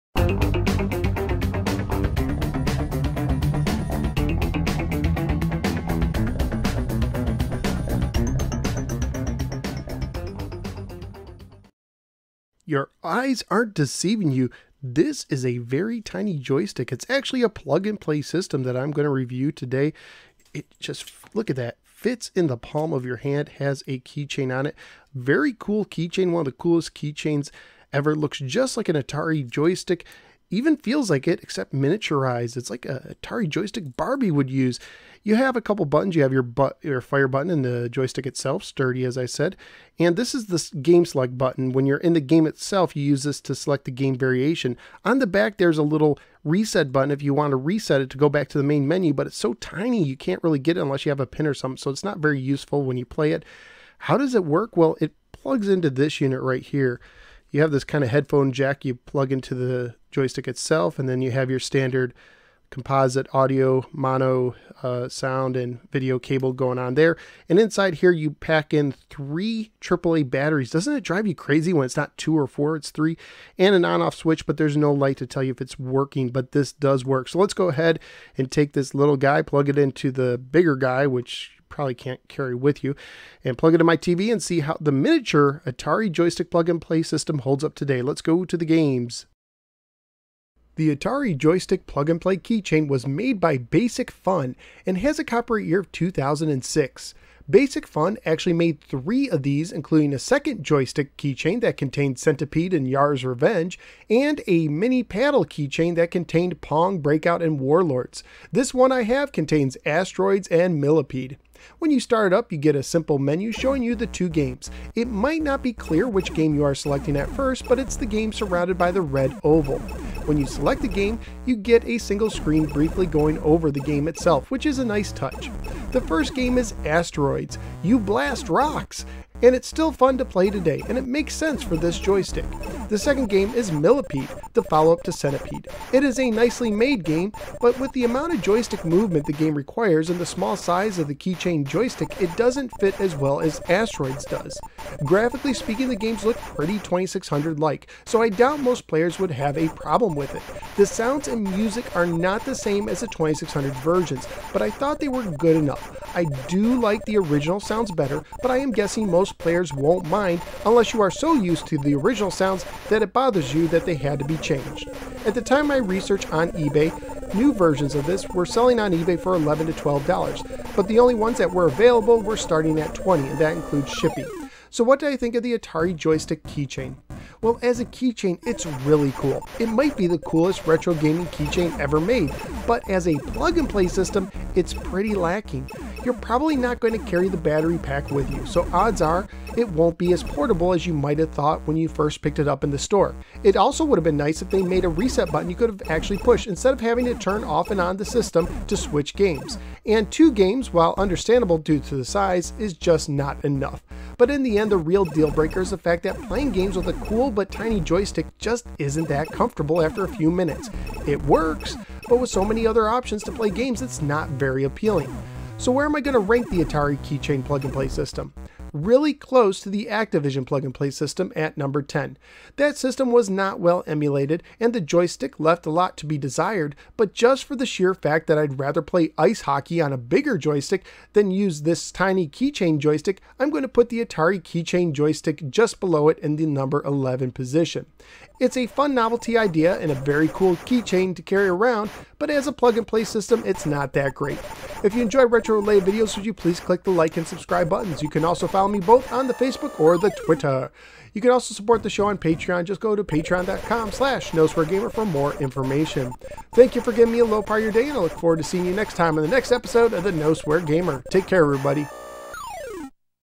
Mini Atari Key Chain Plug n Play System Review - The No Swear Gamer Ep 267
Today's episode covers the Miniature Atari Key Chain Plug and Play system made by Basic Fun.  It contains 2 games: Asteroids & Millipede.